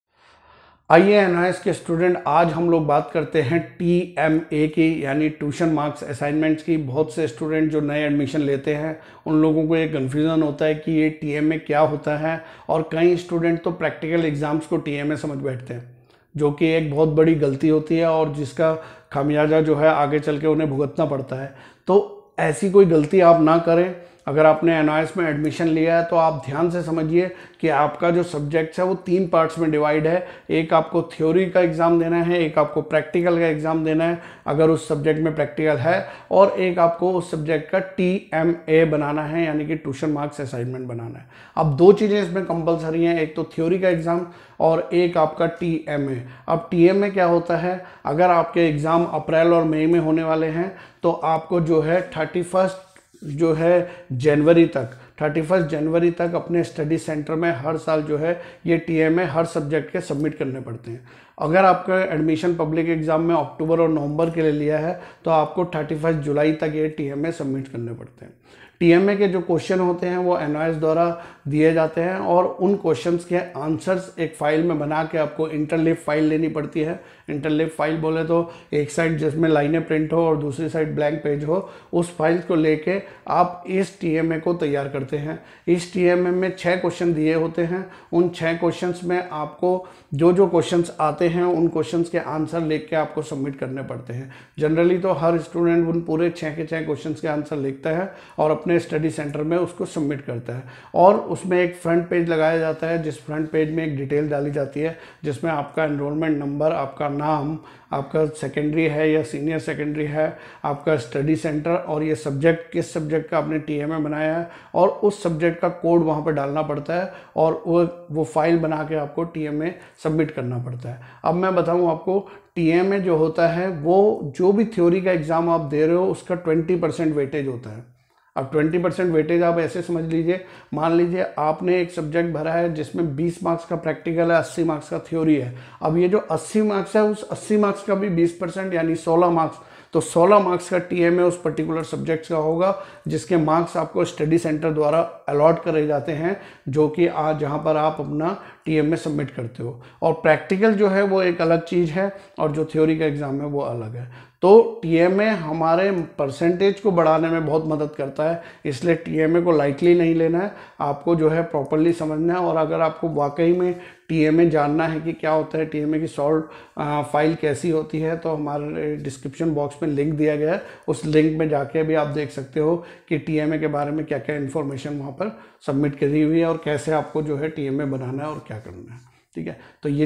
NIOS TMA
What is TMA, Where we have to submit, how can submit TMA, how can TMA prepare?

FOR TMA and other notes, important questions, practical file.

TMA – for OCT/NOV 2020 Public Examination
Class X(Secondary Level)